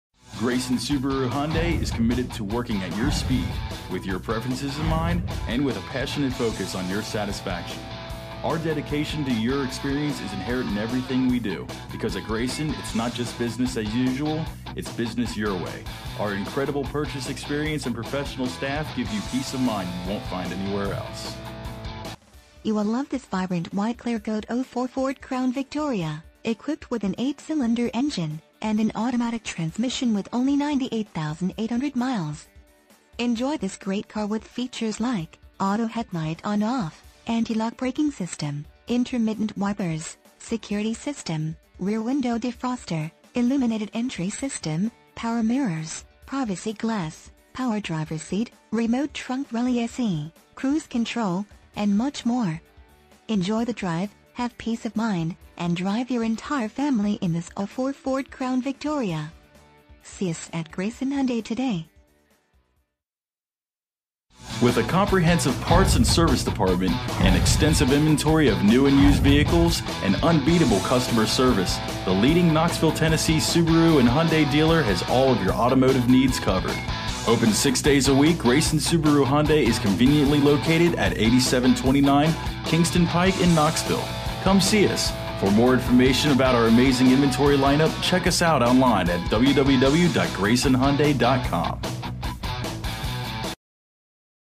2004 Ford Crown Victoria Grayson Hyundai Knoxville, TN 37923
Ford Crown Victoria Check out this Vibrant White Clearcoat Ford Crown Victoria 4 Dr Sedan, equipped with an 8 Cylinder engine and an automatic transmission with.
MILES: 98,800
VIN:2FAFP73WX4X177201

Grayson Hyundai
8729 Kingston Pike
Knoxville, Tennessee 37923